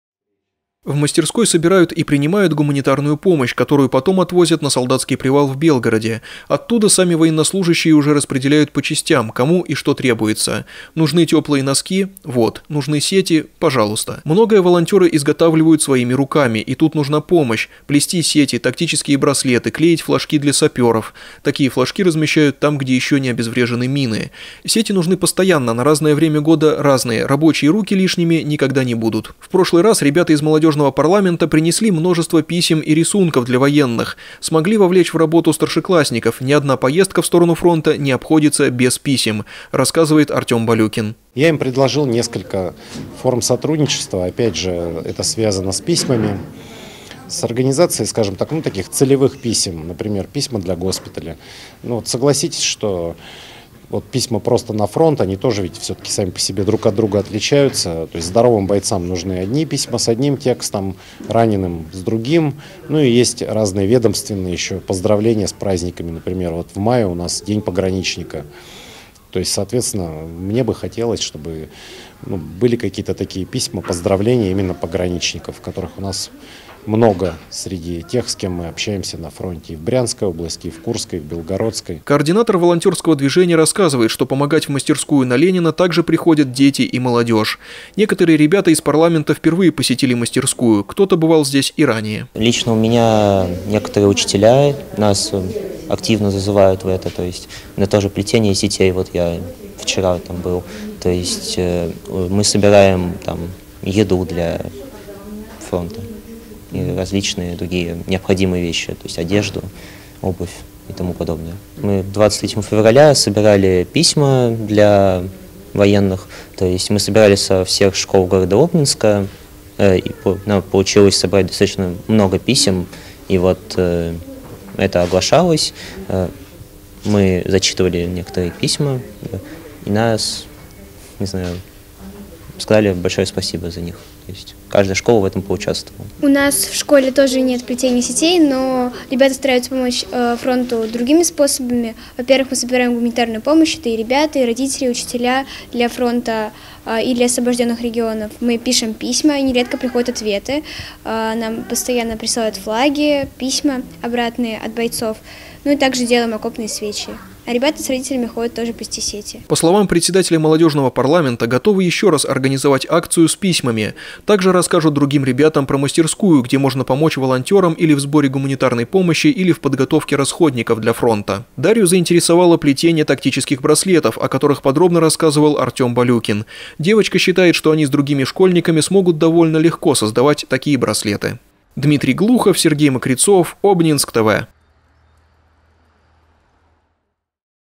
02.04.2024. Обнинский Молодёжный парламент вновь посетил народную мастерскую
Члены «Молодёжного парламента» Обнинска вновь посетили народную мастерскую волонтёров «СВОих не бросаем! 40 RUS» на Ленина, 10. Руководитель проекта Артём Болюкин рассказал ребятам, чем занимаются добровольцы, какую помощь школьники могут оказать мастерской и, соответственно, - военнослужащим на фронте.